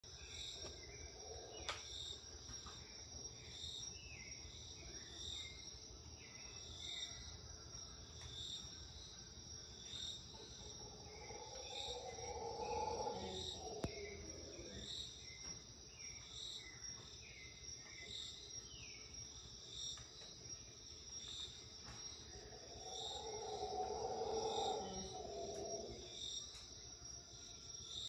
Tranquil sounds in Costa RIca as night approaches day
Listen and enjoy. at Hotel Cerro Lodge between 4 and 5 AM.